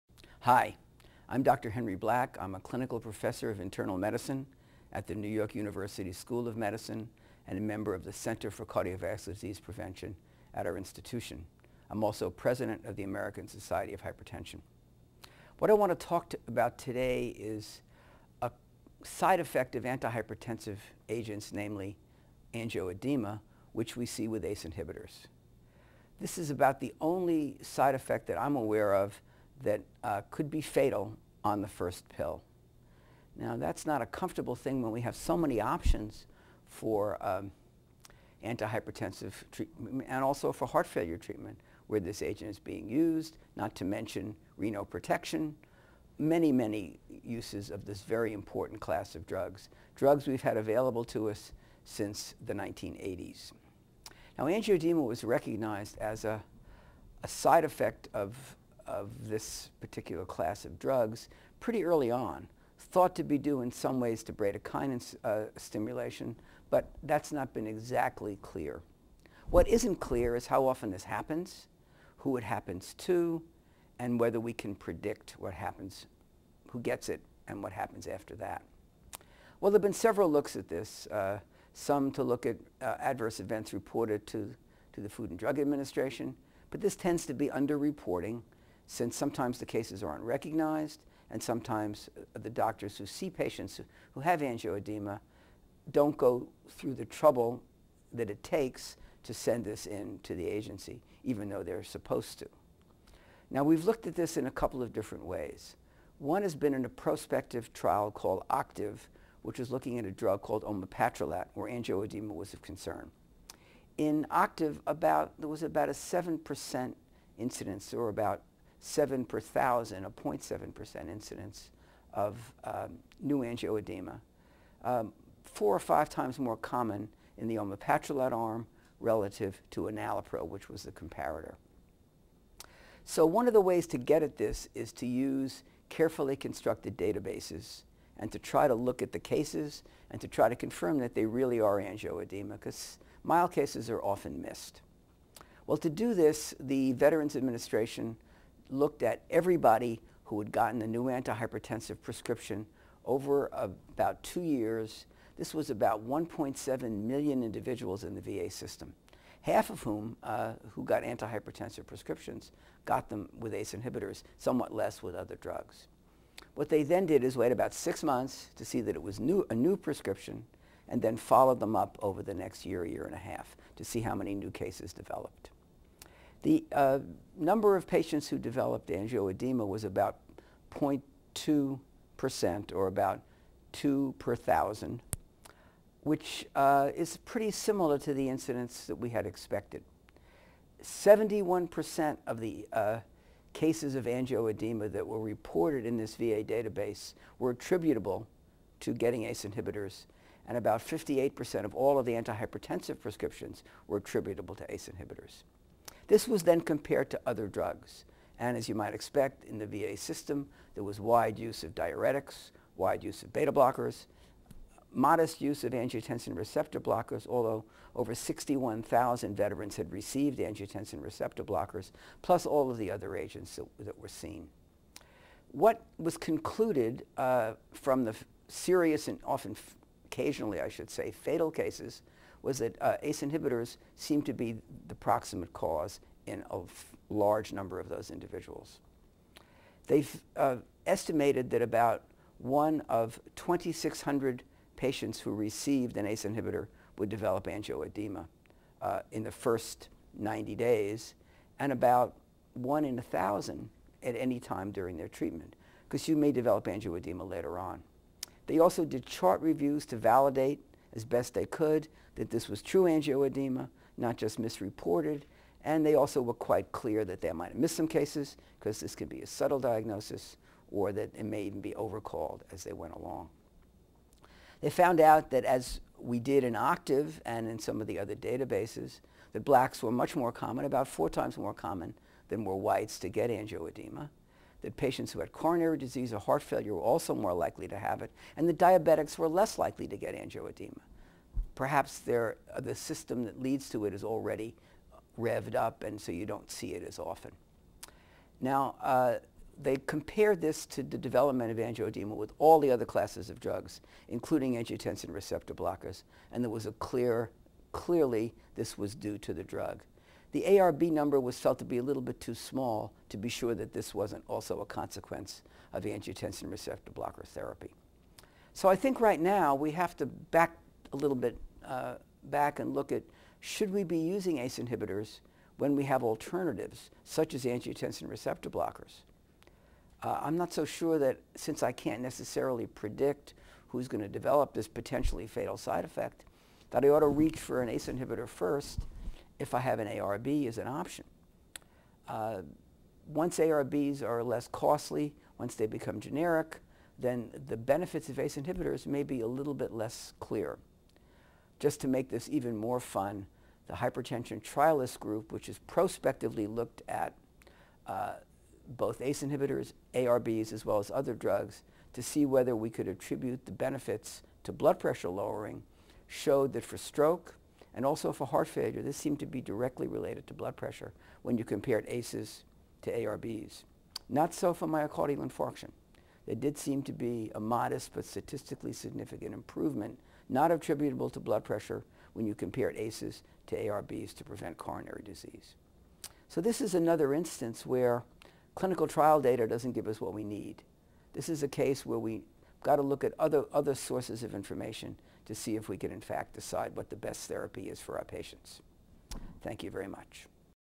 ACE Inhibitors and Angiodema -- Can the Veterans Affairs Database Tell Us Who's at Risk?
Dr. Henry Black says that not all critical information comes from clinical trials. He cites a VA study on angioedema -- the only side effect from an antihypertensive that can be fatal with one pill